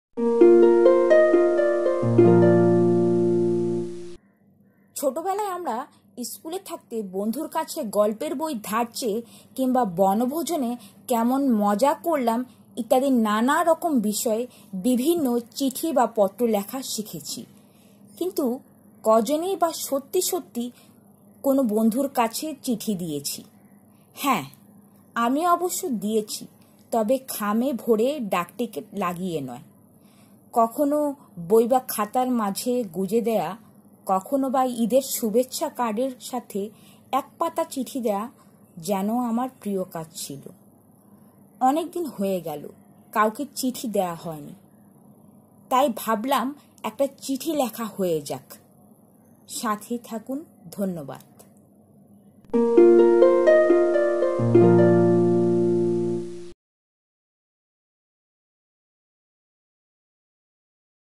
বন্ধুর কাছে খোলা চিঠি আসছে....
ইন্টারনেটের এই যুগে চিঠির প্রতি আগ্রহ বা ভালবাসা কি কমে গেছে? উত্তর "হ্যাঁ" হতে পারে, উত্তর "না" হতে পারে। তবে পুরোনো ছিল বলেই নতুন এসেছে।

বহুদিন পর চিঠি লিখছি। কিছুটা বাস্তবিক, কিছুটা কাল্পনিক। সাথেই থাকুন।

Video by Nahar Al Razi